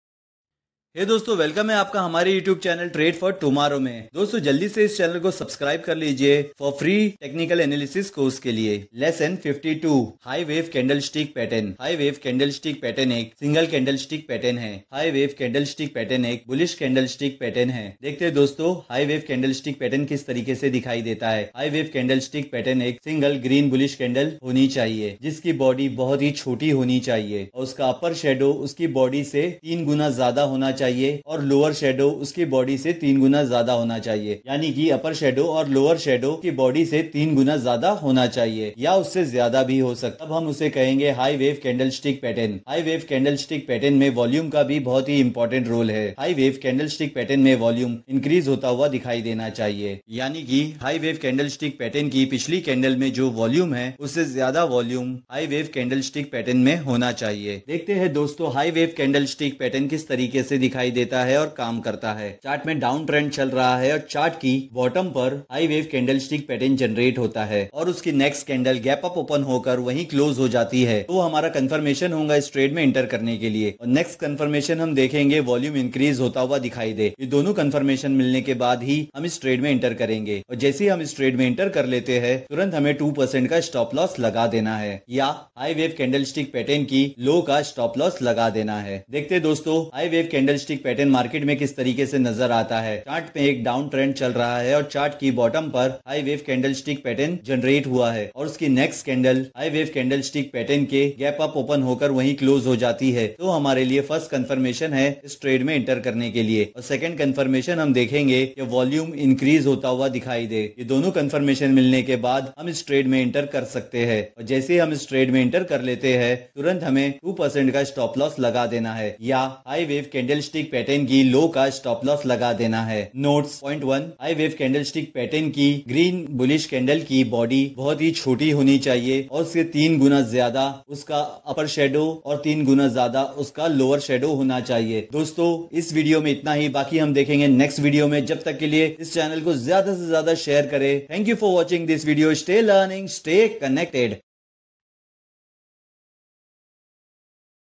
Lesson 52 high wave candlestick pattern (free stock market technical analysis course)
what is high wave candlestick pattern

how to used high wave candlestick pattern